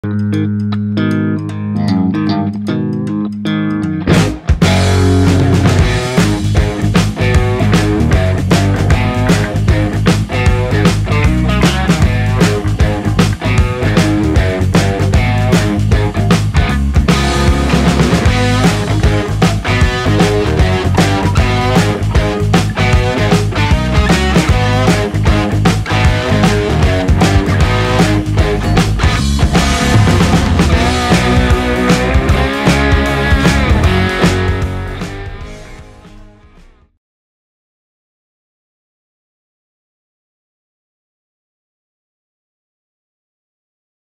RYOBI 18V ONE+ 325 CFM Jet Fan Blower
The RYOBI 18V ONE+ Jet Fan Blower (P21130) features a jet fan design allowing for maximum airflow. Delivering 325 CFM and 100 MPH this blower makes dry leaf clean up a breeze.